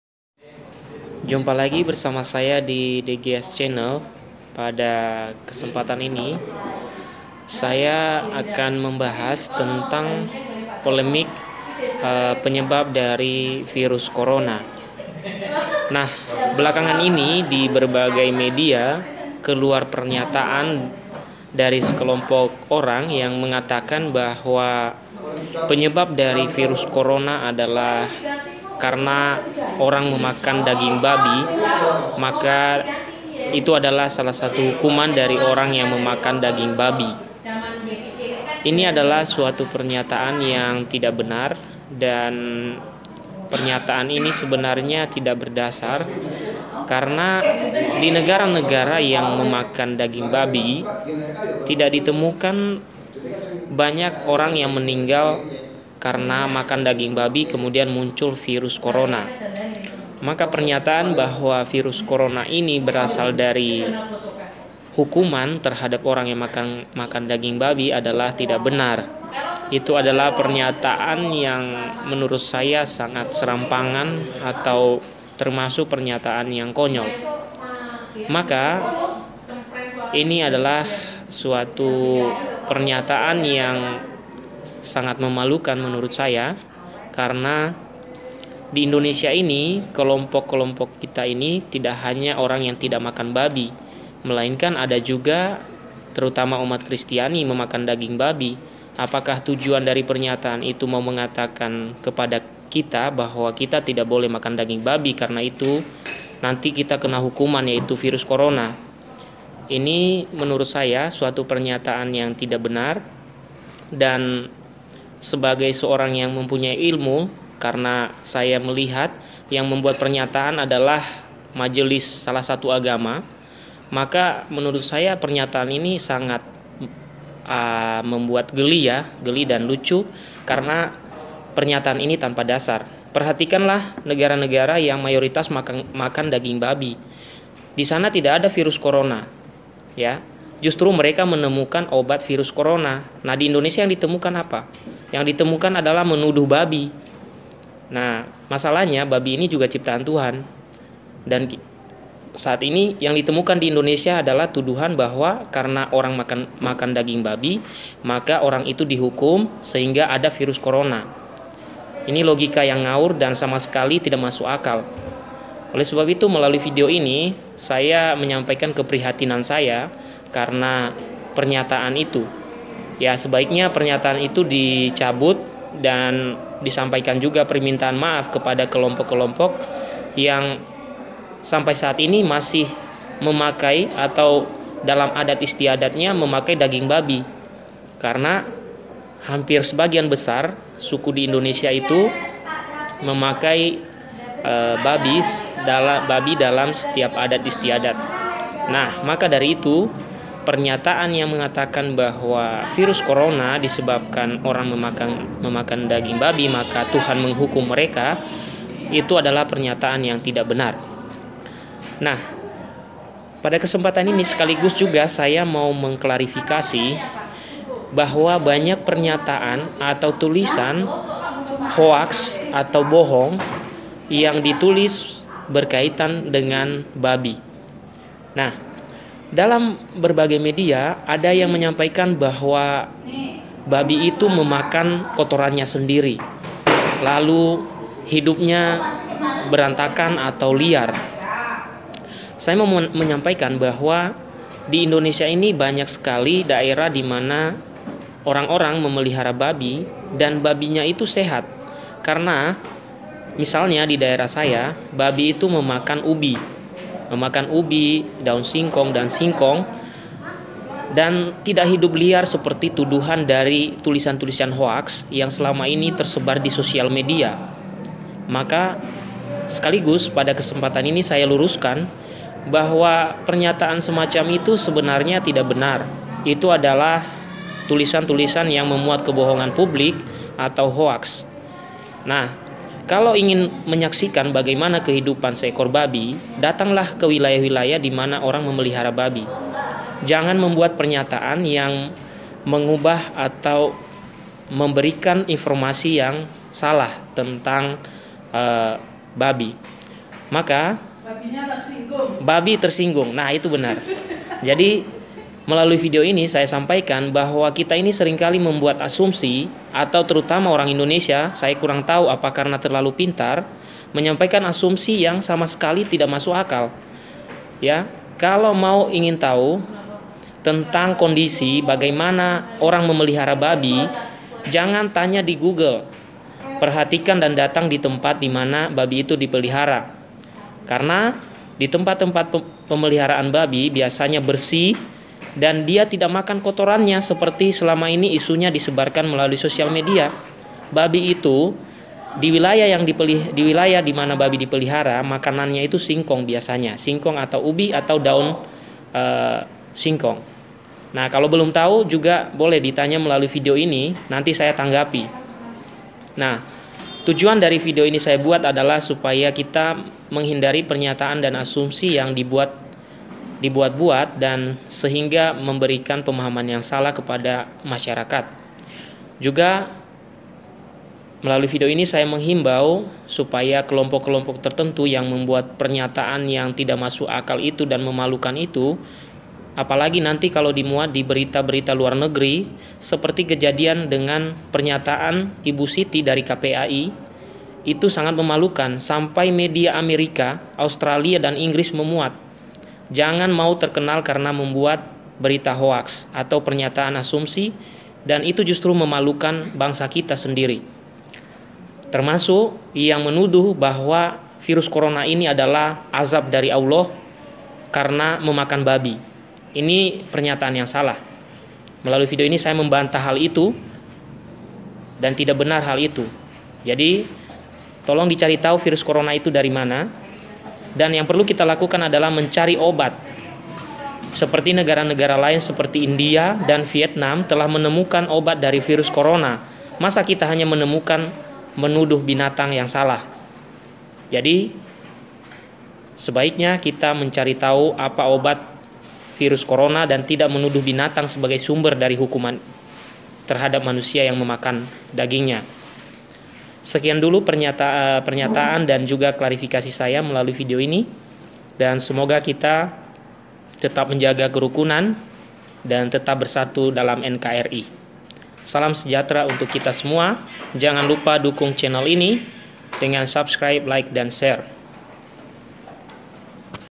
Virus Corona, Azab Bagi Pemakan Daging Babi?. Sebuah Pernyataan Konyol
Menanggapi Pernyataan Bahwa Virus Corona adalah Azab bagi pemakan Daging Babi